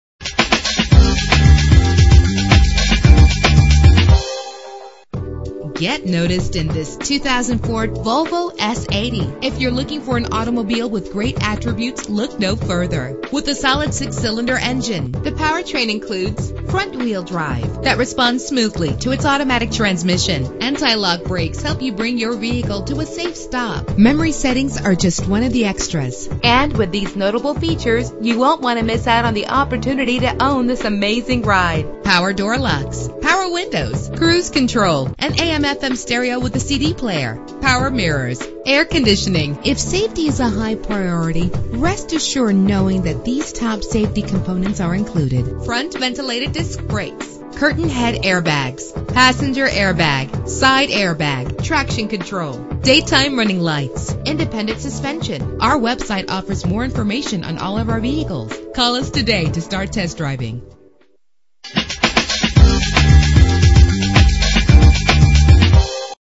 2004 Volvo S80 - Ocala FL
Year: 2004
Make: Volvo
Model: S80
Trim: 2.9
Engine: 2.9 liter inline 6 cylinder 24 valve
Transmission: 4-Speed Automatic
Color: Black
Mileage: 93356
Address:
3440 South Pine Avenue